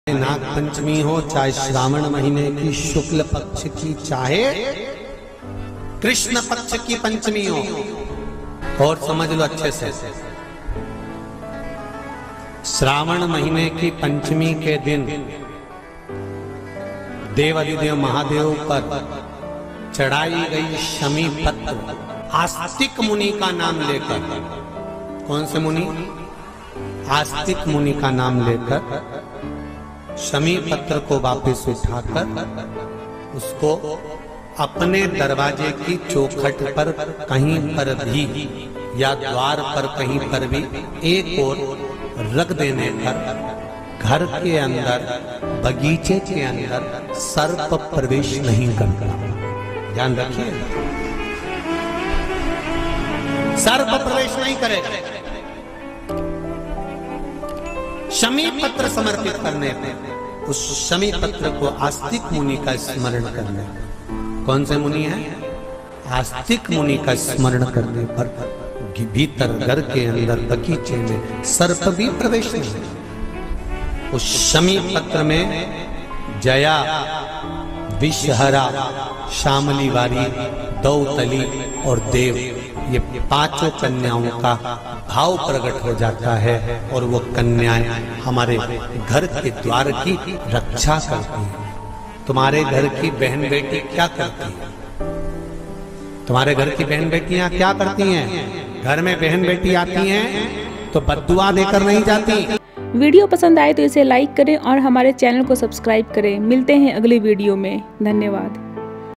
2 अगस्त नाग पंचमी को शमी पत्र को अपने घर में एक ऐसी जगह रख दें घर में बरक्कत कभी नहीं रुकेगी
श्री शिवाय नमस्तुभयं |
बाबा भोले की कृपा का पात्र बने  |
बम बम भोले |
कुबेरेश्वर महादेव |